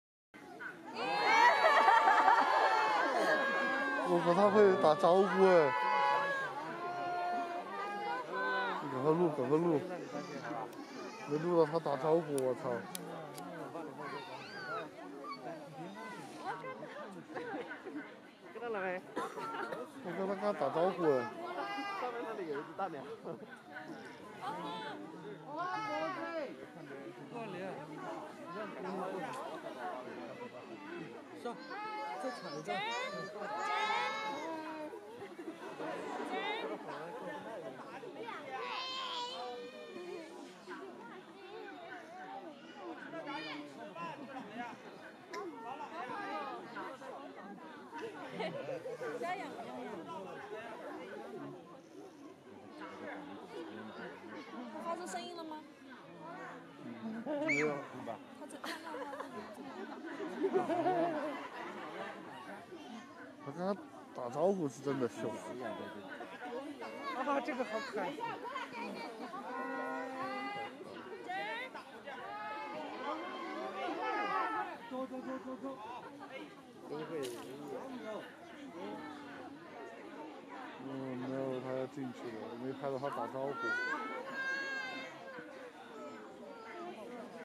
HeHua waves to her fans 😄
影片于2023/1/3（二）拍摄 This video was filmed on January 3 (Tuesday), 2023 at the Chengdu Research Base for Giant Panda Breeding.

🌸HeHua ♂(2020/7/4)
☘️HeYe    ♀(2020/7/4)
     Mother: Cheng Gong (2000/9/11)   Father: Mei Lan (2006/9/6)

🐼Run Yue ♂(2020/6/5)
     Mother: Yuan Run (2012/8/25)   Father: Lou Bao (Born in 2007 in the wild)

🐼Ai Jiu ♂(2020/6/5)
     Mother: Ai Li (2011/7/24)  Father: Gong Zai (2008/7/26)

    The four cubbies lived together in Grandpa Tan's kindergarten after their separation from mom.
    They are lovingly referred to as "the Fab Four".

Pandas born in the same year as HeHua, HeYe, Run Yue, Ai Jiu include:
Yuan Bao 圆宝 in Taipei
FuBao 福宝 in South Korea
Xiao Qi Ji 小奇迹 in Washington
Fuhin 枫浜 in Japan